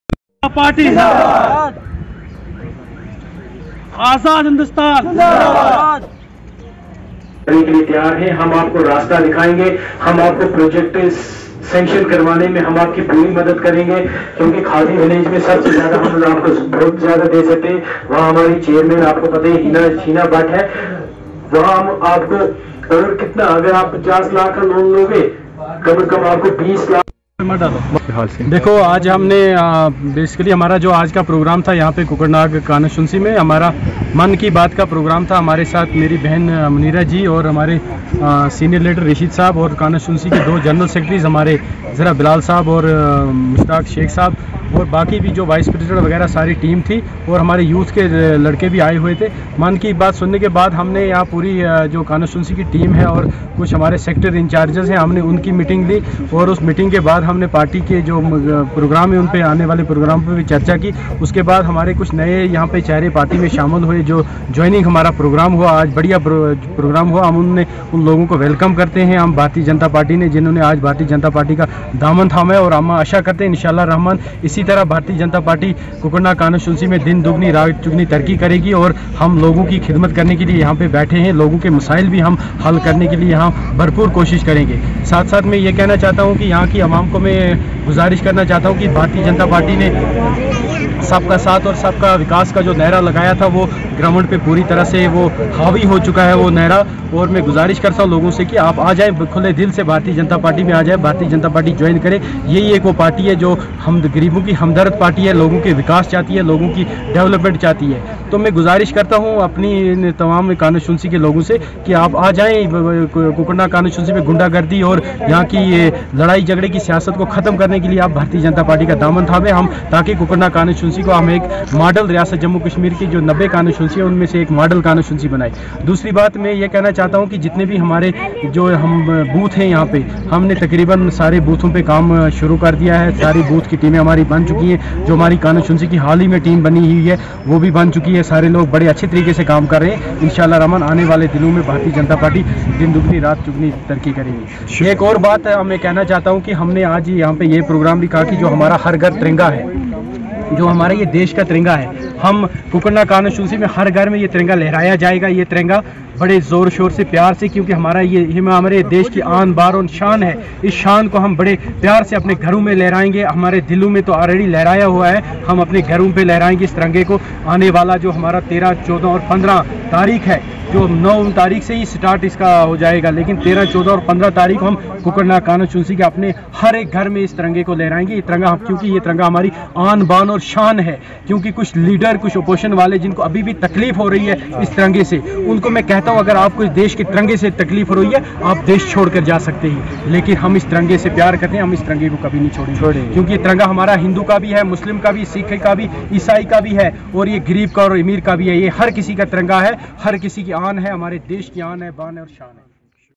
Choudhary Roshan Hussain State General Secretary and BJP Monority morcha state vice president
Choudhary Roshan Hussain State General Secretary and BJP Monority morcha   state vice president  Miss muneera tantary Media incharge abid Shah present in joining programme at kokernag budjet accommodation  incharge Kokernag ST Constituency organize a programme of  Man ki baat at Kokernag, He was accompanied with Senior Leader Ab Rasheed, Constituency General Secretaries Bilal Ahmed and Mushtaq Ahmad Sheikh  Malik and large number of party workers participate in the meeting regarding